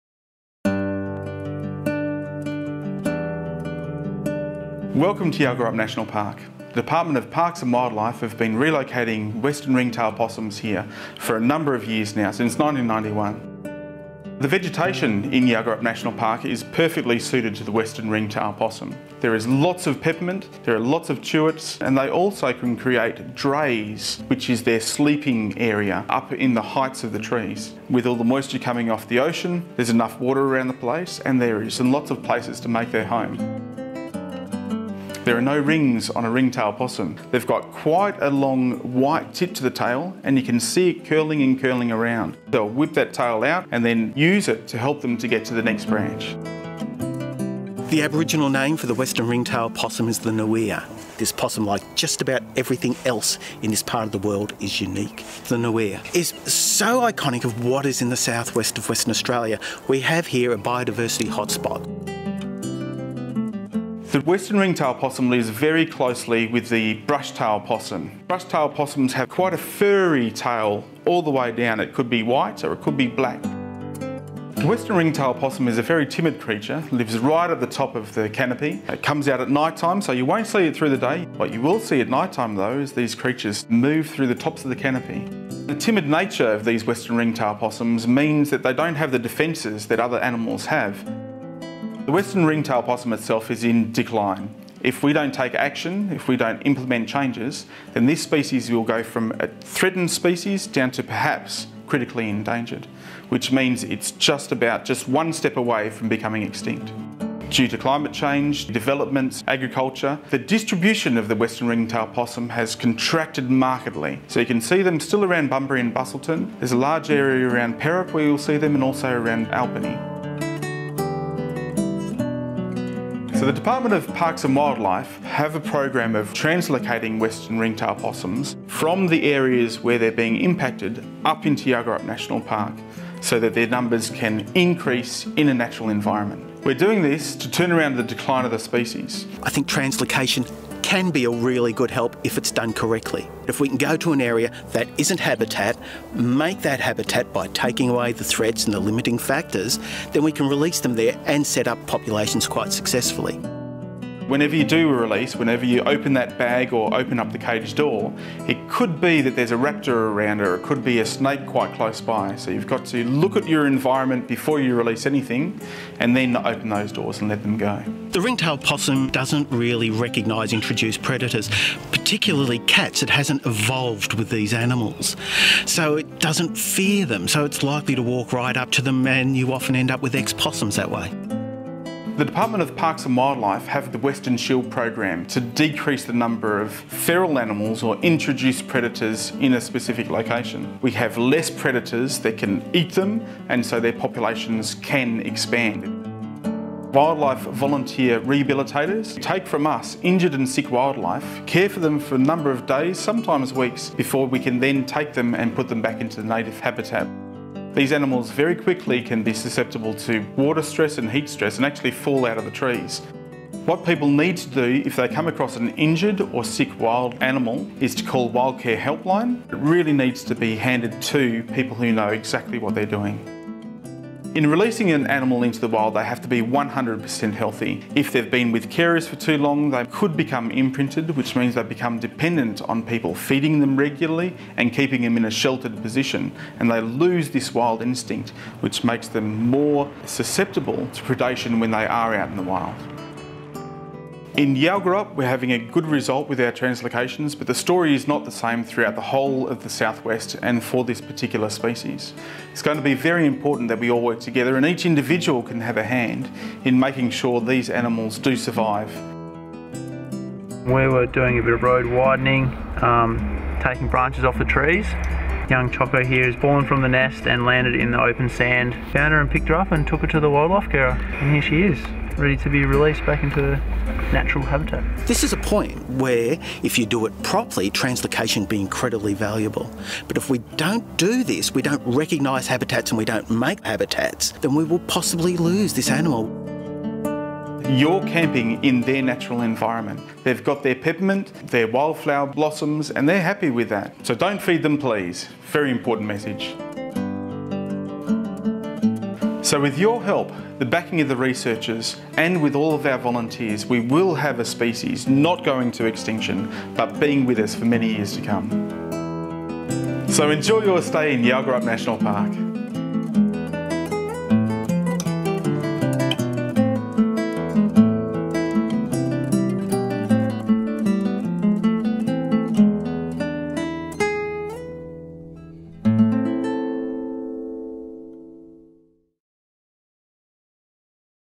Relocating the Western Ringtail Possum
See how the Department of Parks and Wildlife is working to save the threatened western ringtail possum in Western Australia, through its translocation program.